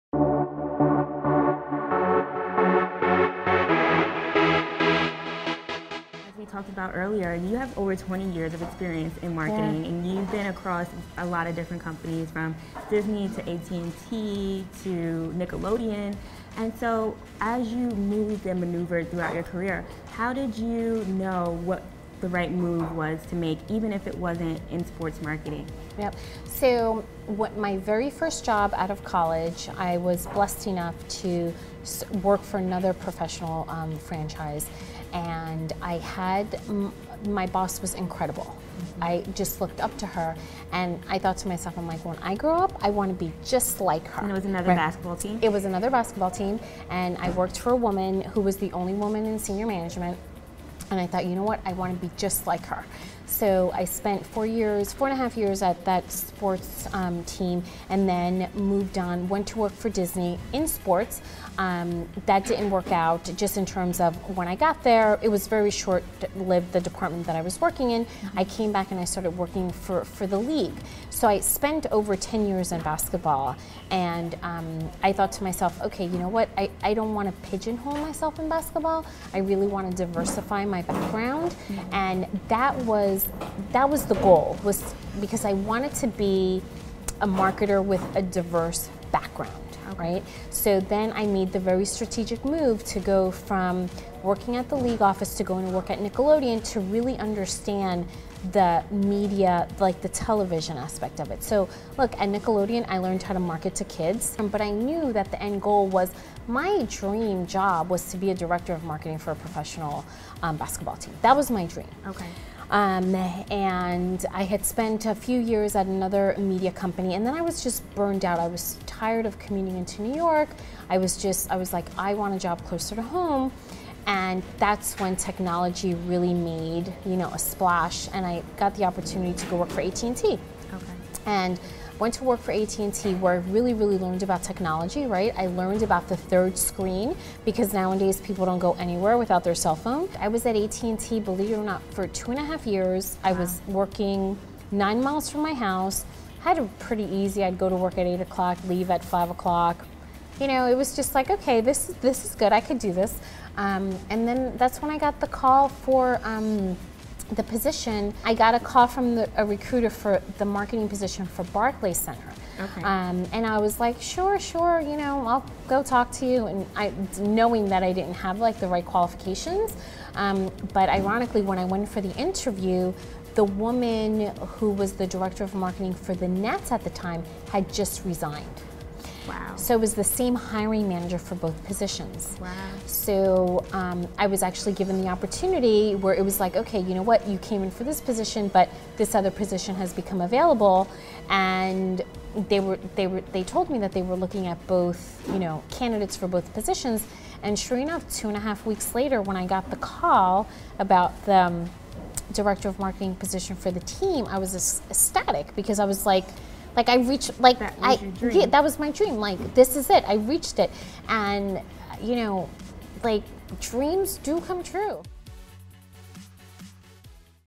Bonus Clip - Elisa Padilla on Getting The Call For Her Dream Job
HerAgenda.com sat down for an interview about the career journey of Elisa Padilla, SVP of Marketing for Brooklyn Nets/Barclays Center. This is a bonus clip from the main interview which you can find